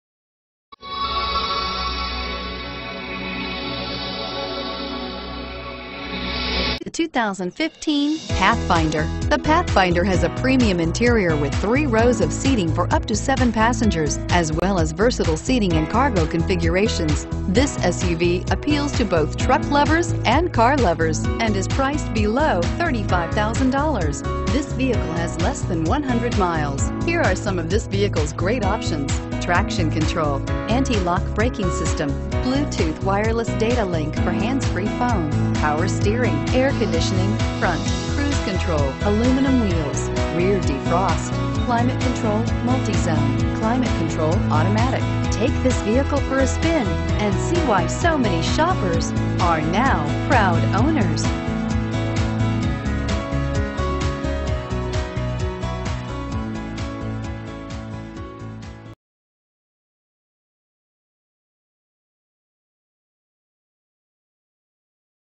2015 Nissan Pathfinder Morrow GA C691384
2015 Nissan Pathfinder

Pick up this Arctic Blue Metallic 2015 Nissan Pathfinder, available today at Nissan South Morrow. Featuring Automatic transmission, it has 5 miles. This could be the one you've been searching for.